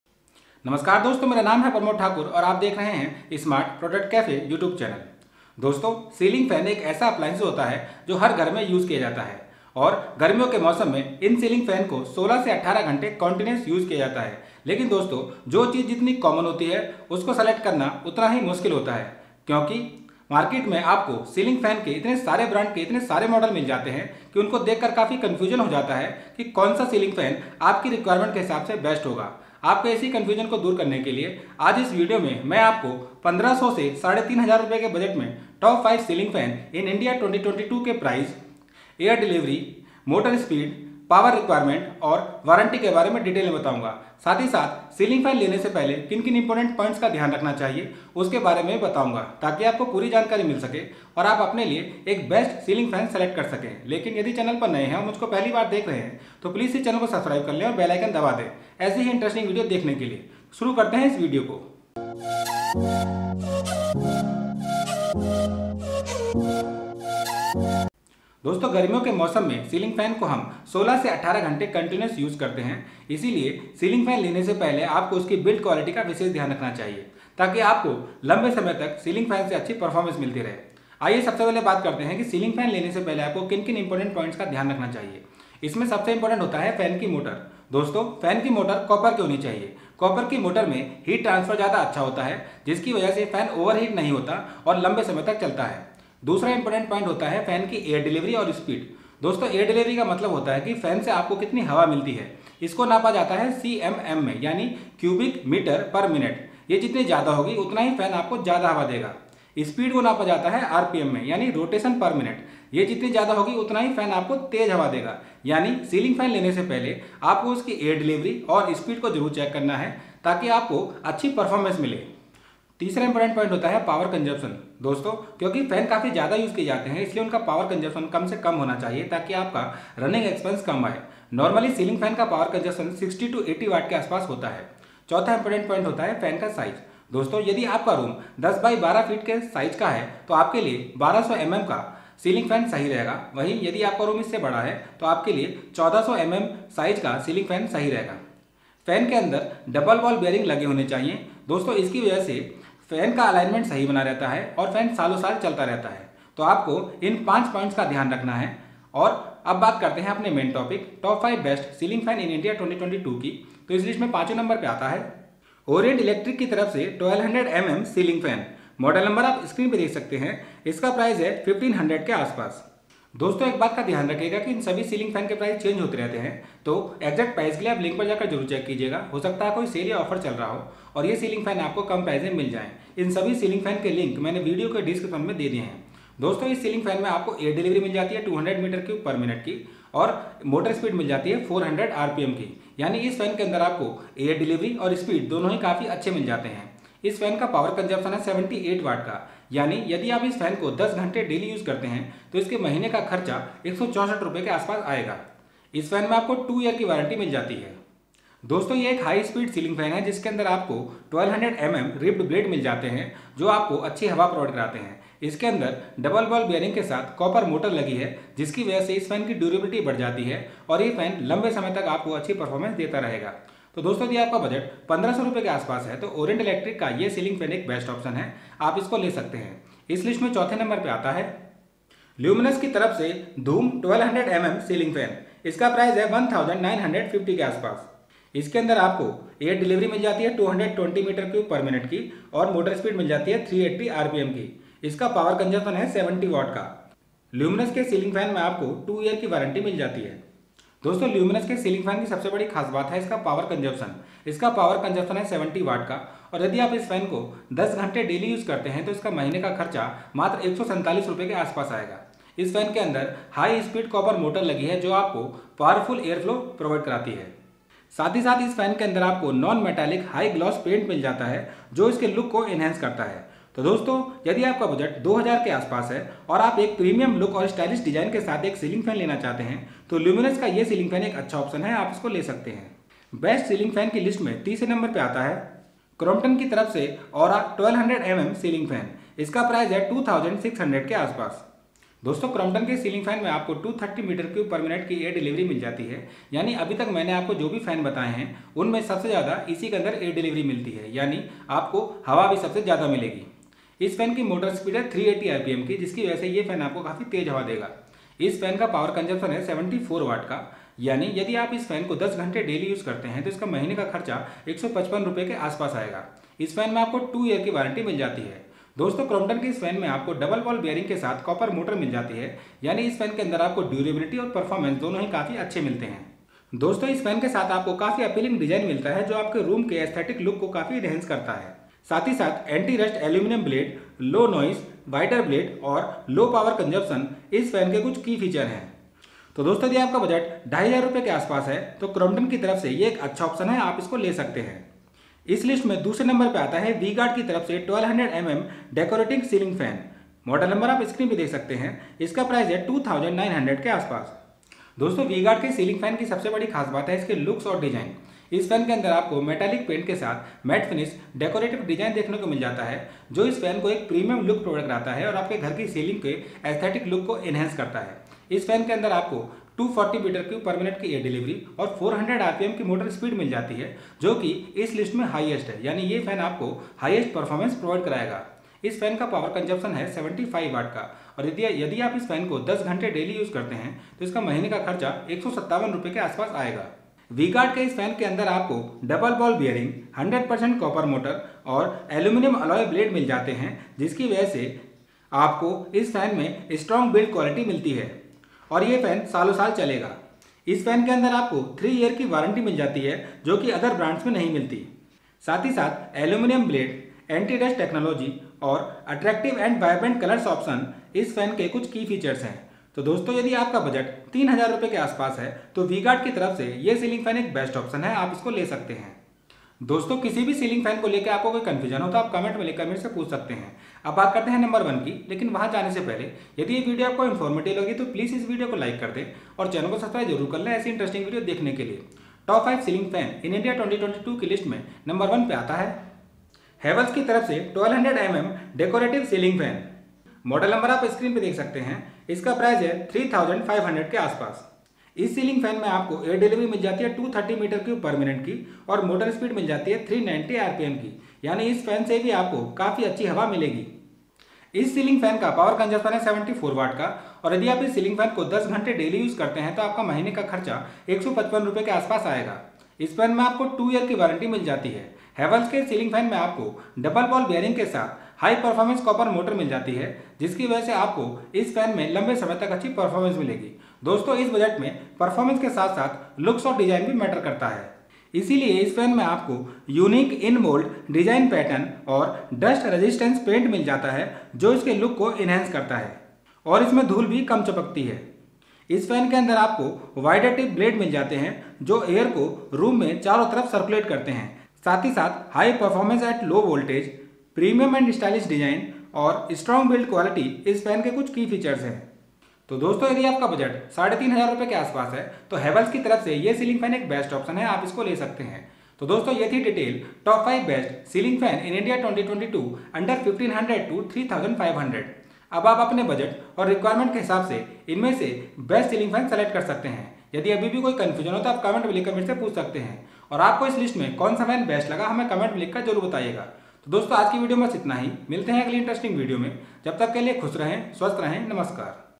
Top 5 Ceiling Fan in India 2022 (Under 1500 - 3500) | Best Ceiling fan 2022 in India
Hello Friends, In this video, I have explained about Top 5 Ceiling Fan in India 2022. These are the best Ceiling fan in India 2022. I have selected top 5 best Ceiling fan 2022 on the basis of performance, Quality, Reliability & customer feedback. I have also explained about the Price, Air delivery, Motor Speed, Power consumption & warranty so that you will select the best Ceiling fan 2022 in India available in the market. I have also explained, Important points to consider before buying a Ceiling fan. I am uploading the video on this channel on regular basis related to Home appliances & Kitchen appliance.

1. Havells Enticer 1200mm Decorative, Dust Resistant High-Speed Ceiling Fan

2. V-Guard Exado Pro Decorative Ceiling Fan with Anti-Dust Technology

3. Crompton Aura 1200 mm (48 inch) High Speed Decorative Ceiling Fan

4. Luminous Dhoom 1200mm 70-Watt High Speed Ceiling Fan

5. Orient Electric Apex-FX 1200mm Ceiling Fan

You Can also follow on Instagram